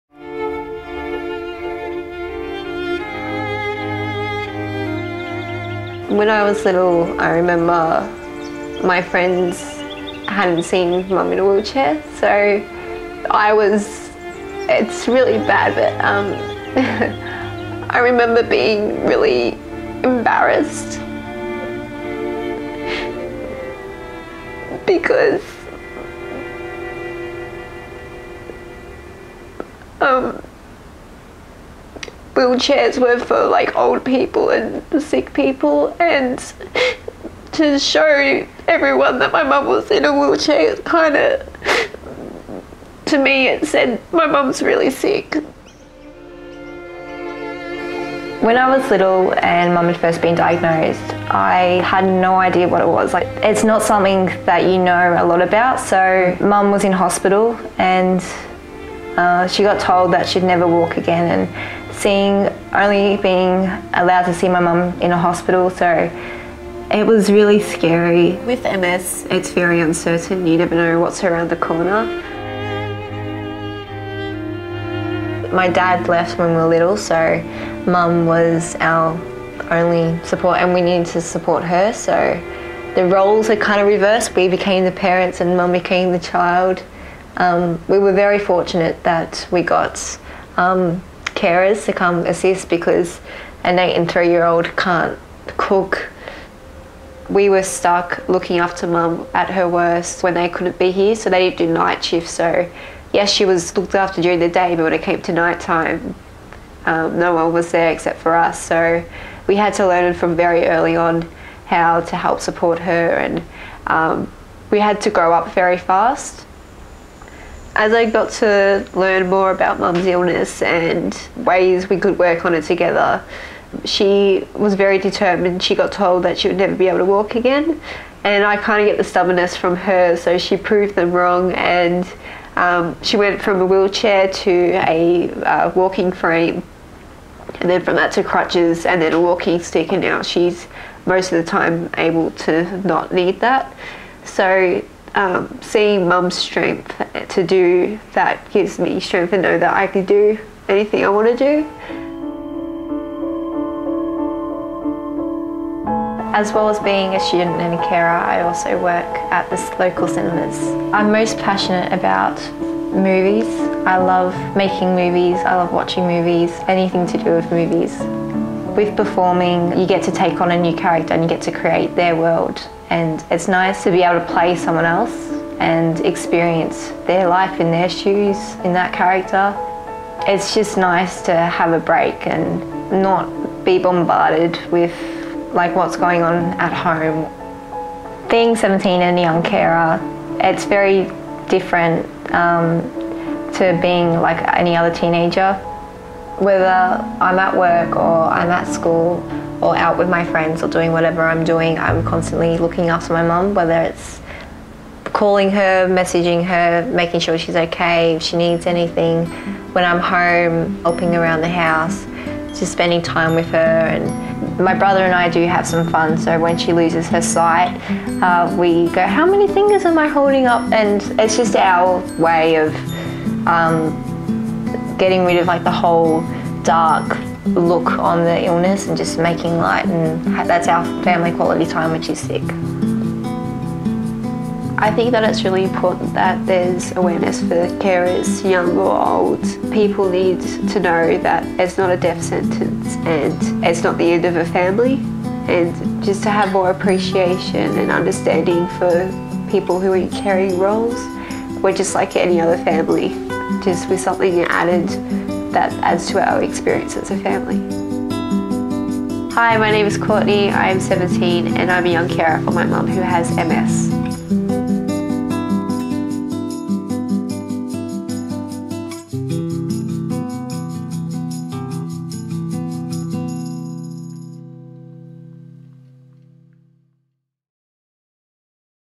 Care Aware: Courtney's Story
Hear Courtney's story. Courtney and her brother are young carers for their mother. Courtney juggles her caring role with school and part-time work.